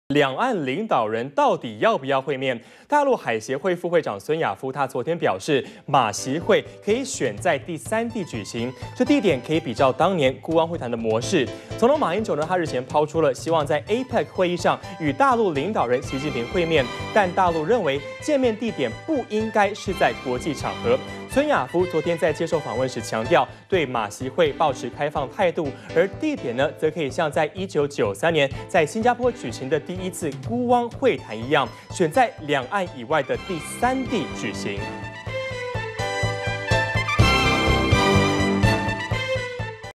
【2014.03.04】談馬習會 孫亞夫:可選在第三地 -udn tv
兩岸領導人，到底要不要會面？大陸海協會副會長孫亞夫，昨天表示，馬習會可以選在"第三地舉行"，地點，可以比照當年，辜汪會談的模式。完整內容 , FB , Twitter , G+